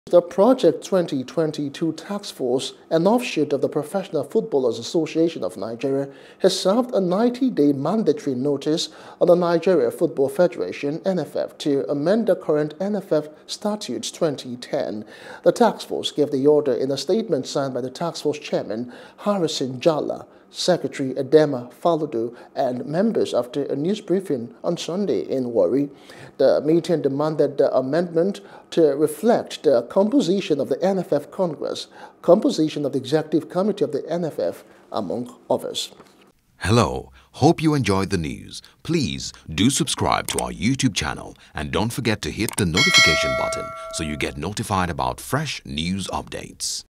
Statutes Amendment: Professional Footballers Taskforce Serves NFF 90 Day Notice | SPORTS
The Project 2022 Taskforce, an offshoot of the Professional Footballers Association of Nigeria (PFAN), has served a 90-day mandatory notice on the Nigeria Football Federation (NFF) to amend the current NFF Statutes 2010.